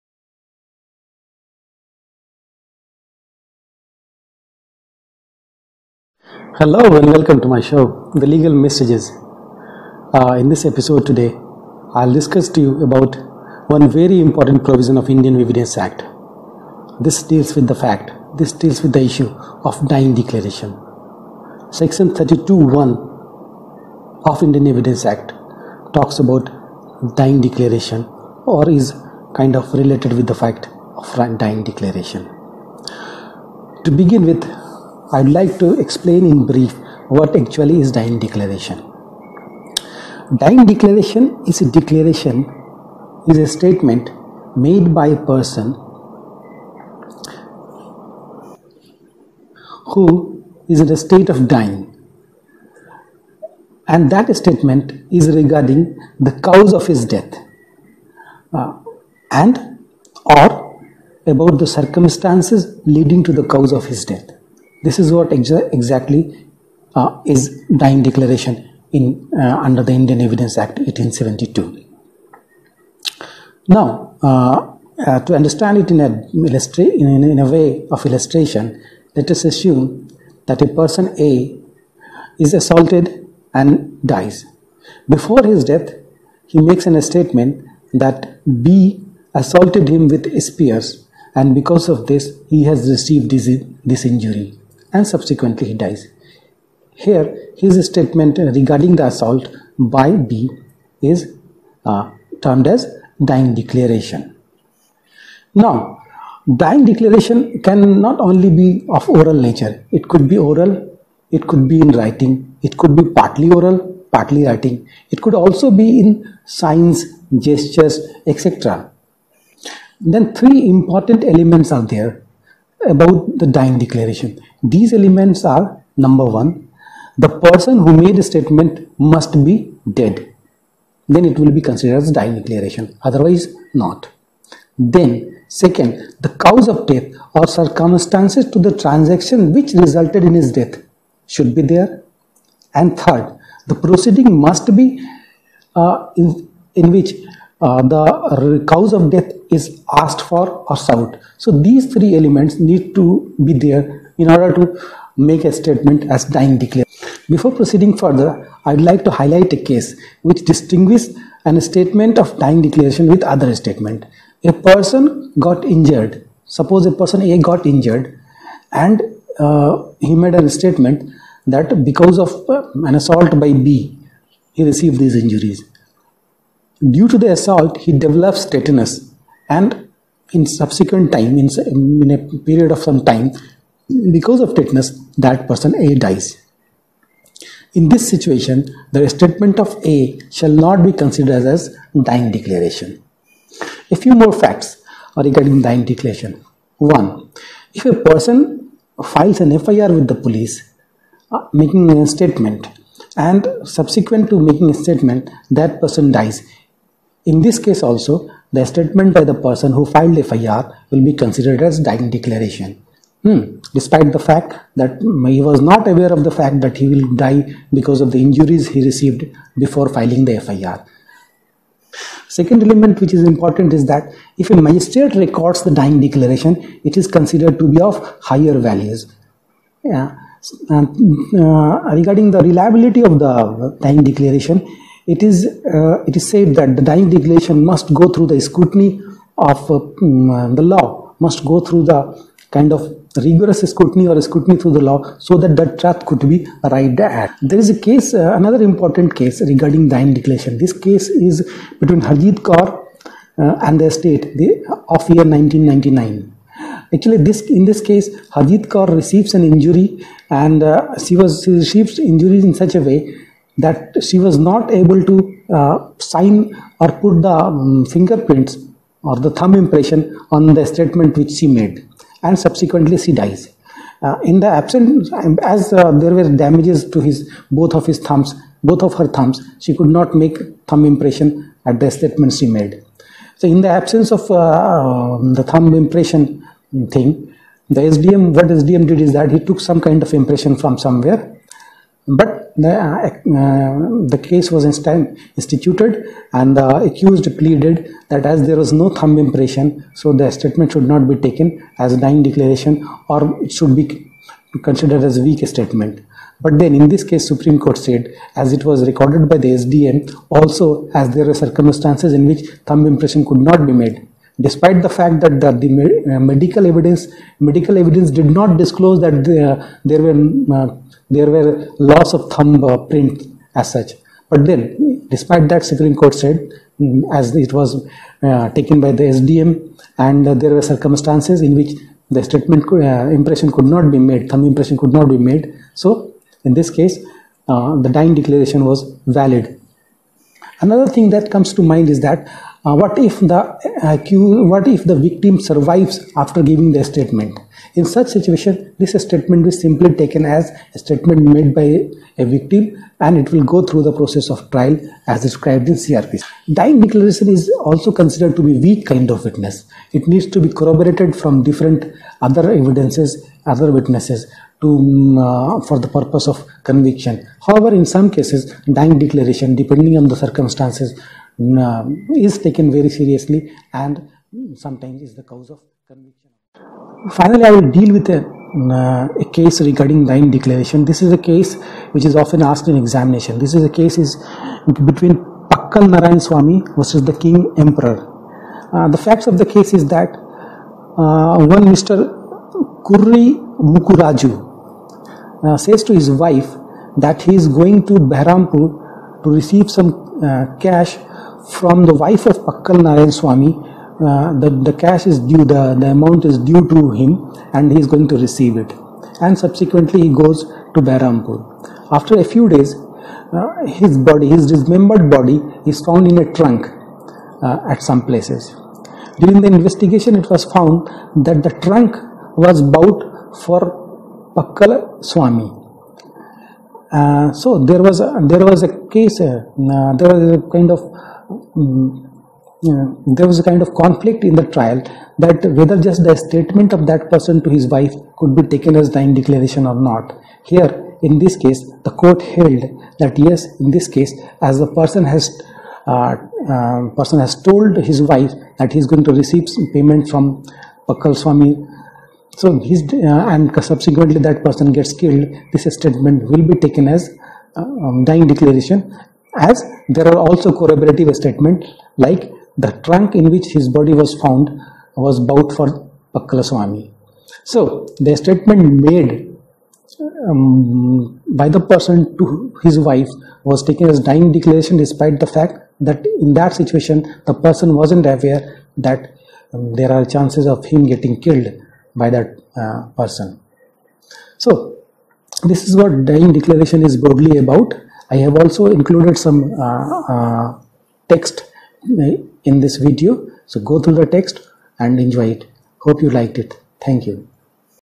Indian Evidence Act: Sec 32(I) - Dying Declaration.
Indian Evidence Act : Sec 32(I) , Dying Declaration. What constitute dying declaration. Dying declaration could be written, oral, in signs. Dying declaration a weak evidence? Case of Harjit Kaur Vs State regarding dying declaration. Elements of dying declaration. Can FIR be treated as dying declaration? Statement made by someone who dies after making it. Is suicide statement, a dying declaration?